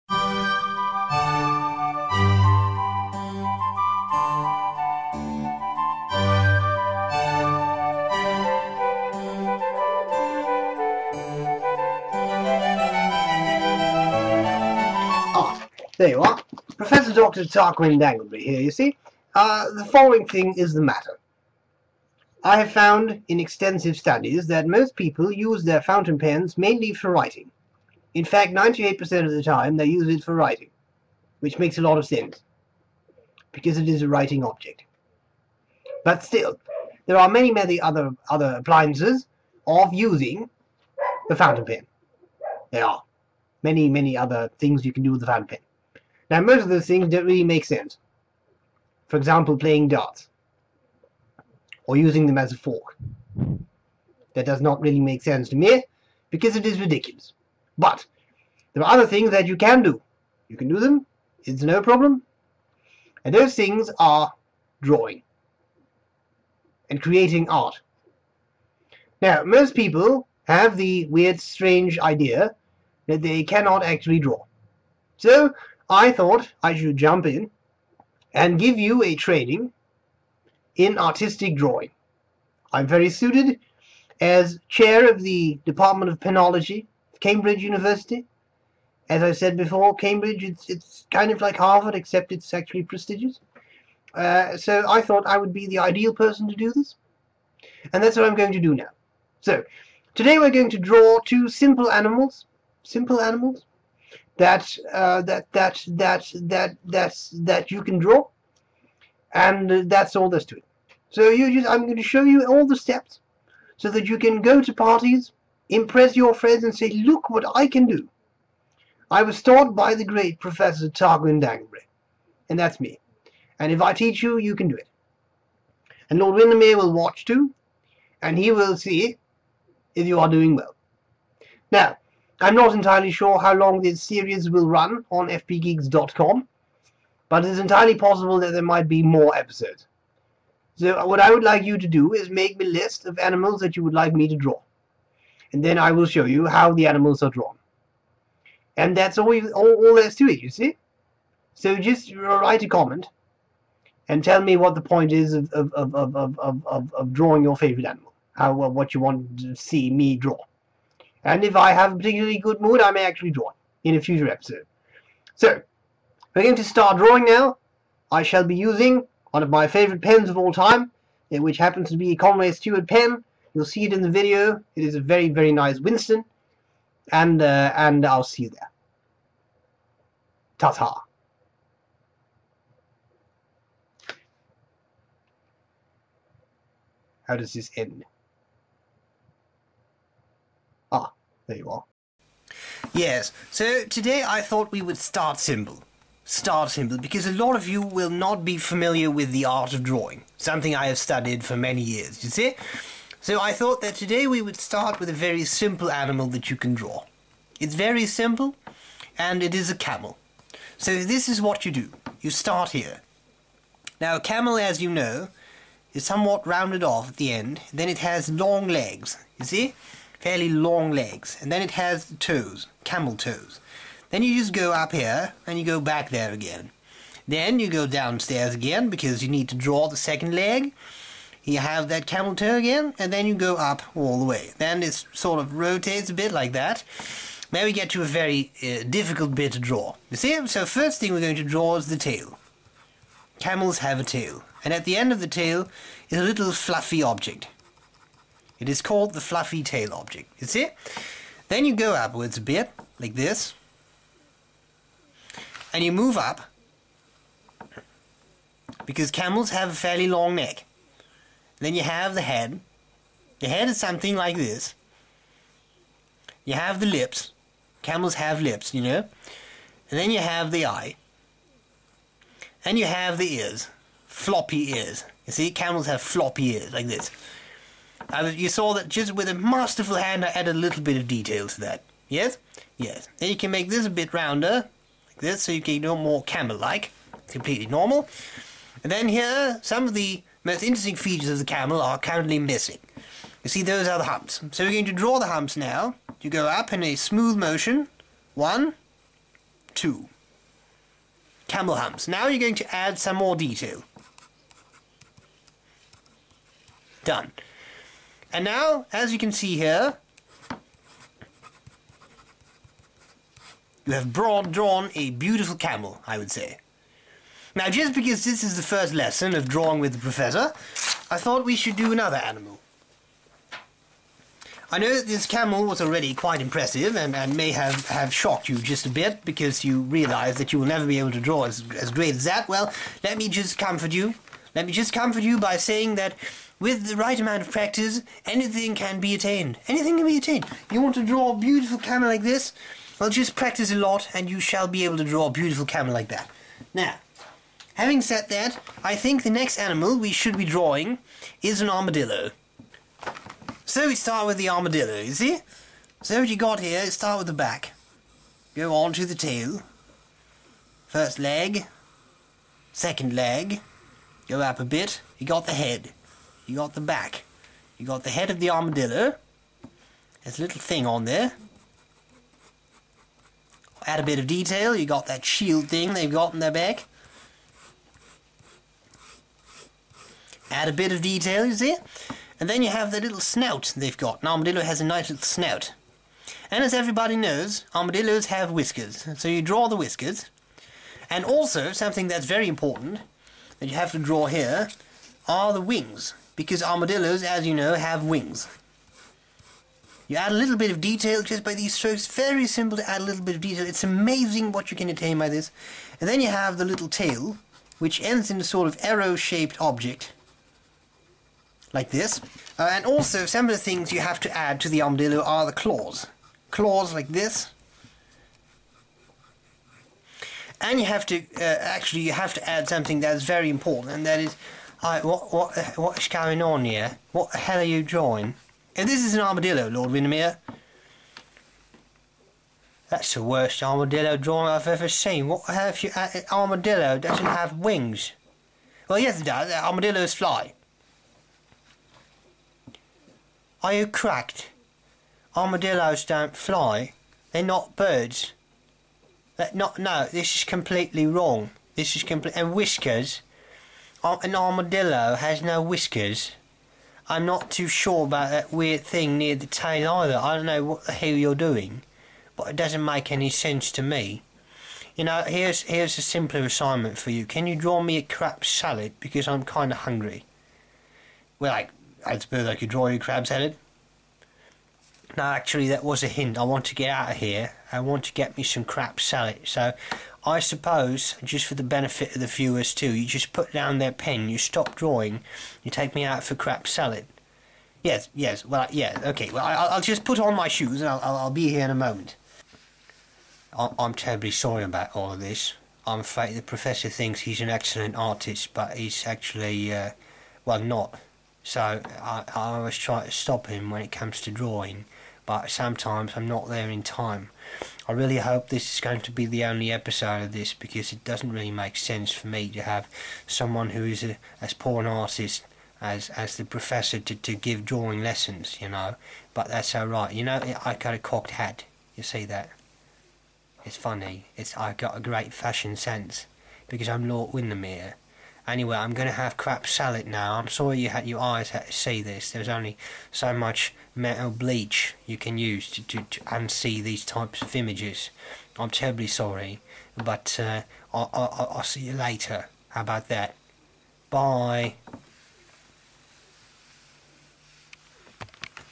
Drawing with the Professor Episode I
Prof.dr. Tarquin Danglebury, Chair of Penology at Cambridge University, kindly provides us with drawing lessons. In today's episode, he demonstrates how to draw a camel and an armadillo.